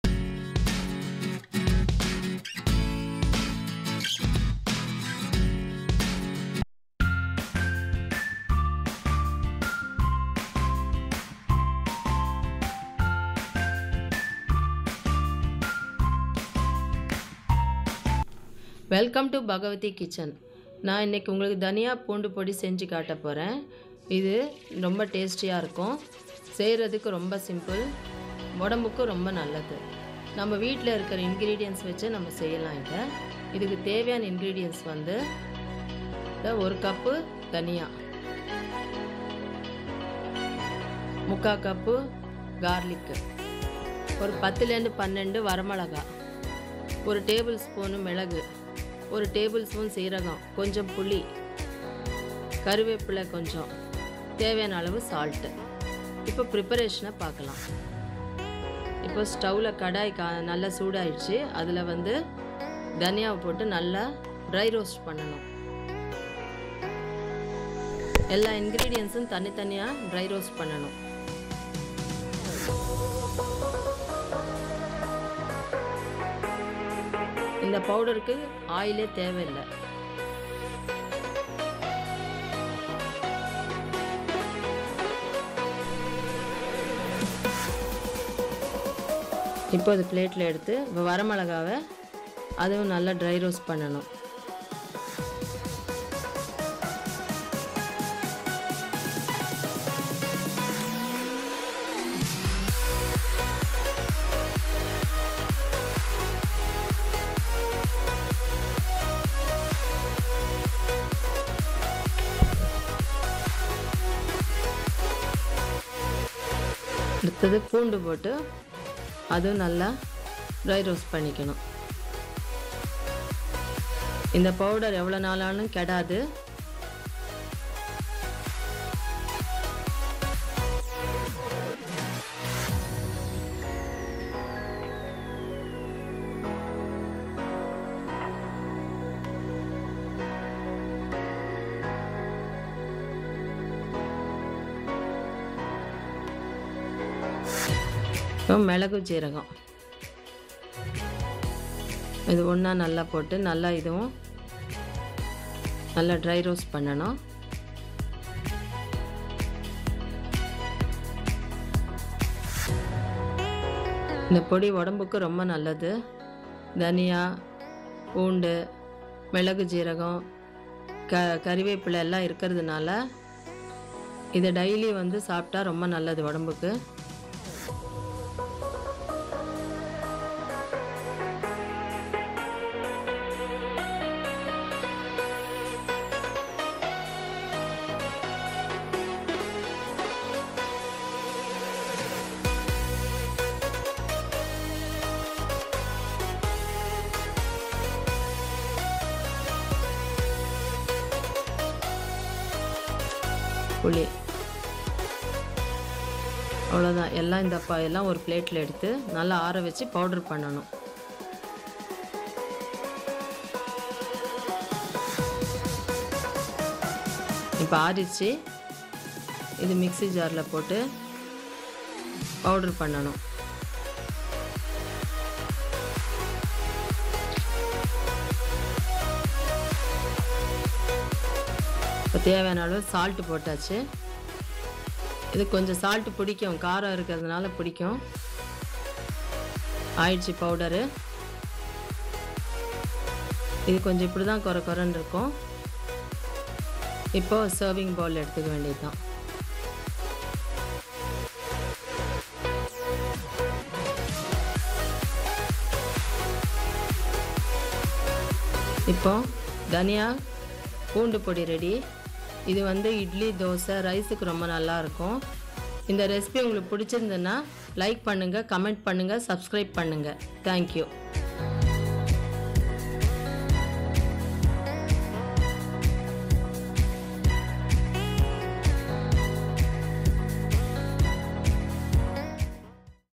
How to make Garlic Coriander Powder( பூண்டு தனியா பொடி) at home?
Bhagavathy Kitchen - Garlic Coriander Powder(பூண்டு தனியா பொடி) in 4K UHD and Dolby Audio.

Benefits of Garlic:
1.The Blood Purifier
2.Cold and Flu Prevention
3.Prevention of Heart Disease
4.Anti-bacterial and Anti-parasitic
5.Cancer Prevention

Vitamin A 0% Vitamin C 52%
Calcium 18% Iron 9%
Vitamin D 0% Vitamin B-6 60%
Vitamin B-12 0% Magnesium 6%

Benefits of Coriander:
1.Coriander lowers bad cholesterol (LDL) and increases the levels of good cholesterol (HDL).
2.A very good food for digestive system, coriander promotes liver functions and bowel movements.
3.Coriander is good for diabetes patients. It can stimulate the insulin secretion and lower the blood sugar levels.
4.he fat soluble vitamin and antioxidant- Vitamin A, protects from lung and cavity cancers.
5. It's a very good herb to promote the nervous system. It can stimulate the memory.
6.Coriander helps those suffering from anaemia. Coriander contains high amounts of iron, which is essential for curing anemia.

Vitamin A 134% Vitamin C 45%
Calcium 6% Iron 9%
Vitamin D 0% Vitamin B-6 5%